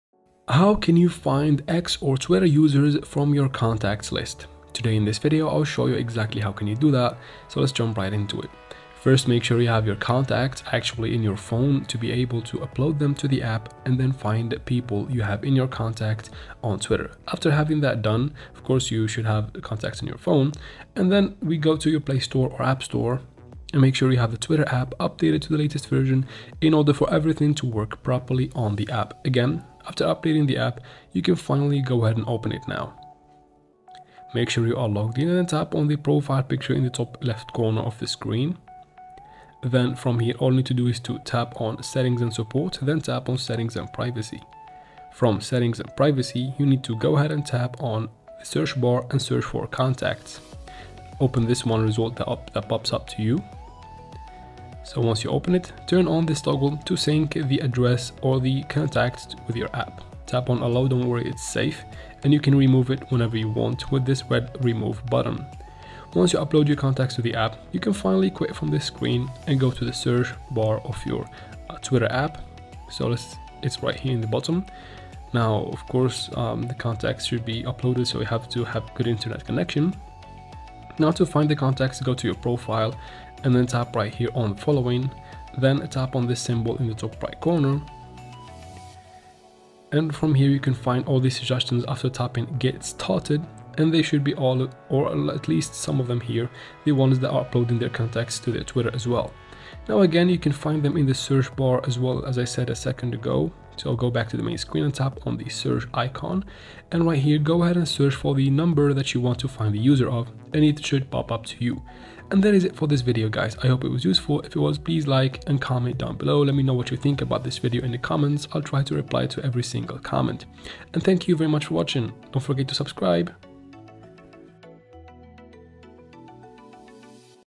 How to Find X (Twitter) Users From Contact List - 2024
Do you want to know how to find X (Twitter) users from contact list? Then this is the video for you! I will show you how to find out X (Twitter) users from mobile contacts list on iphone and android.

This is a step-by-step tutorial on how to find out X (Twitter) users from mobile contacts in 2024. You can learn how to find X (Twitter) users from phone contacts on iphone and android, so don't worry.